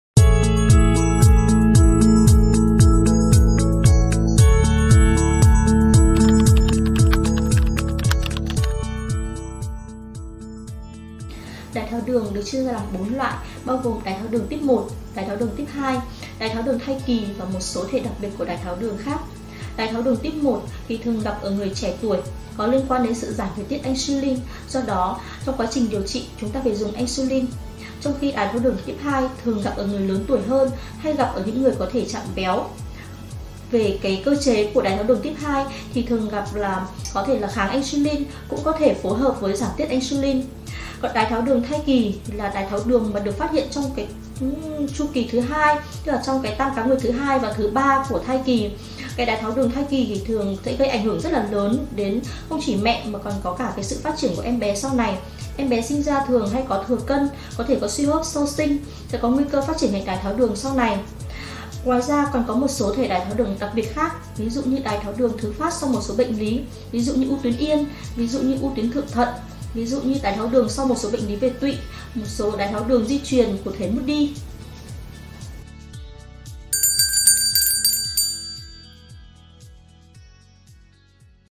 MẮC ĐÁI THÁO ĐƯỜNG TYPE NÀO NGUY HIỂM HƠN?
Việc xác định phân loại bệnh đái tháo đường sẽ phục vụ công tác khám chữa bệnh nhanh chóng. Vì vậy hãy cùng MEDLATEC tìm hiểu có những type đái tháo đường nào? Cái nào nguy hiểm hơn nhé!